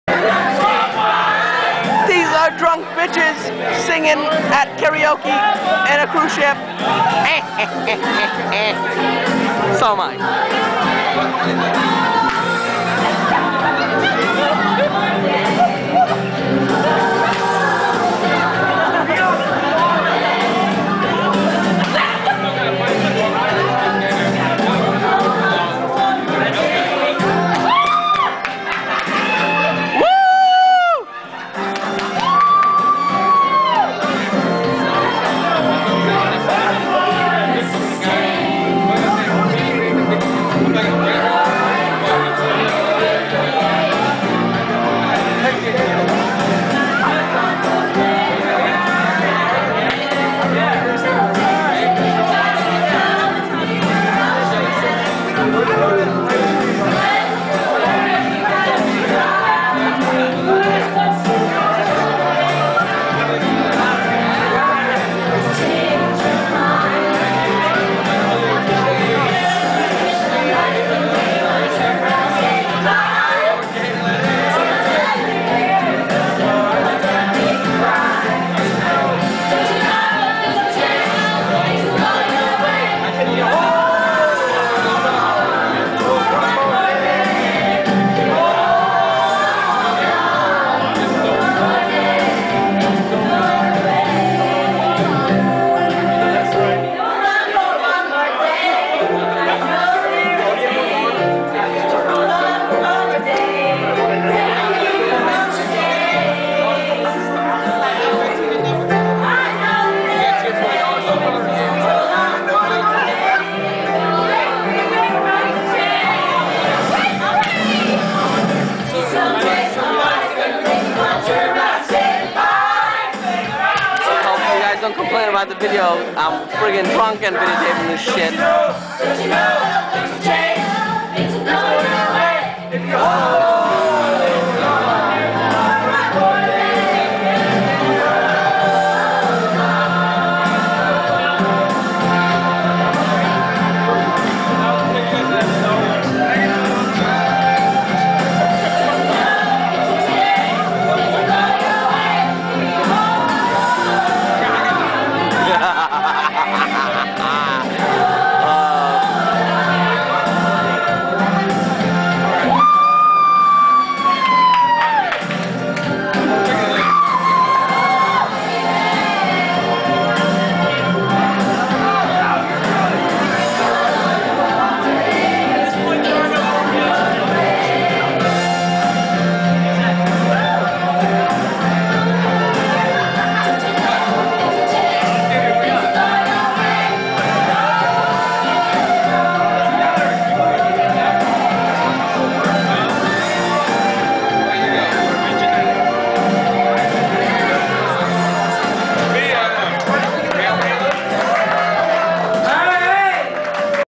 carnival cruise mexico Luli sings
Lourdes sing Karioki while drunk with random women!